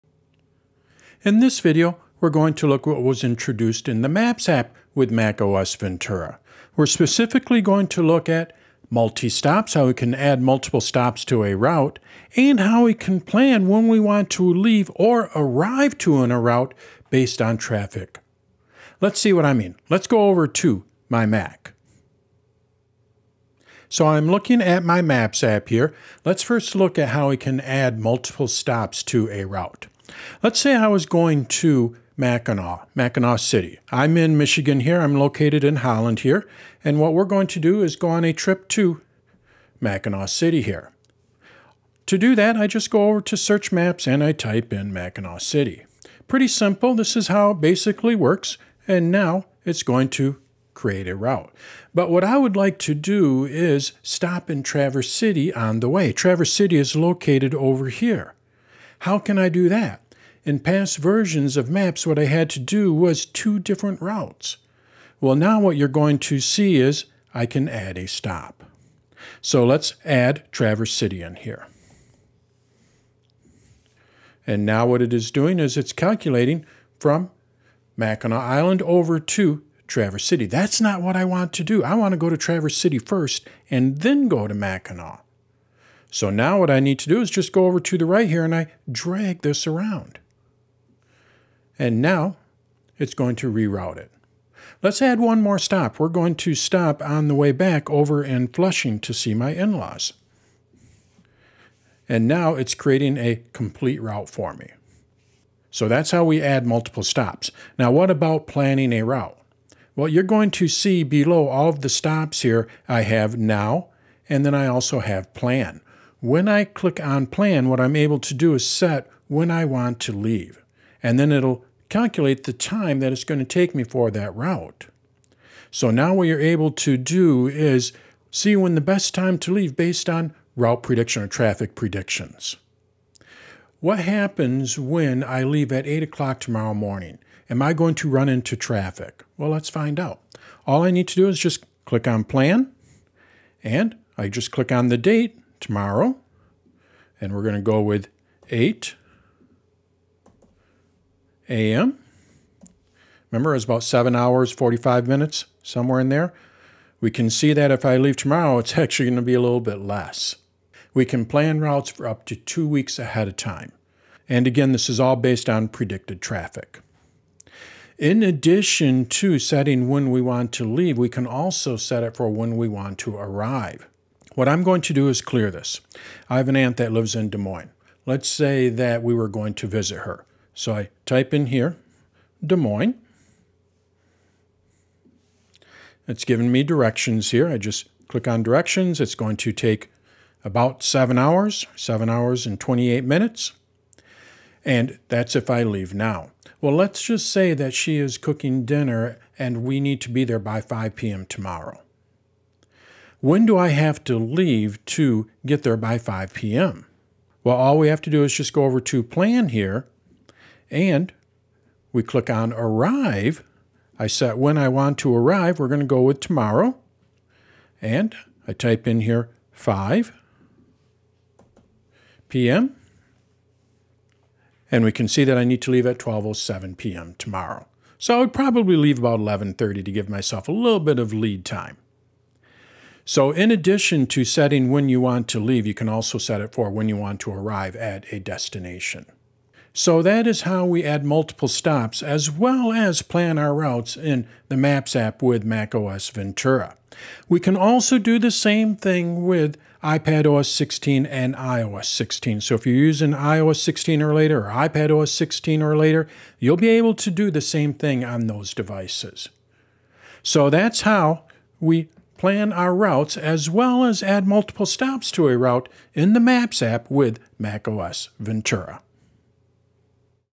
Learn How To Add Multiple Stops to Routes in the Maps App on the Mac
Learn how to add multiple stops to the Maps app on the Mac, as well as how to use predictive travel time to set when you should leave on a trip.

In macOS Ventura, Apple added the ability for users to add multiple stops to a route, and rearrange those stops. We can also have the Maps app suggest when we should leave for a trip if we want to arrive at a specific time. This suggested time is based on predictive traffic. Learn about how to add multiple stops to a route and use predictive traffic to see when you should leave, in this video for for the Maps app on the Mac.

Chapters:
00:00 - Introduction
00:26 - How to Add Multiple Stops to a Route
01:14 - Rearrange Stops in the Maps App
01:47 - Planning A Route with Time
02:13 - Set when you should Leave on a Trip
02:47 - Set When you should Arrive to a Destination
03:59 - Summary and Conclusion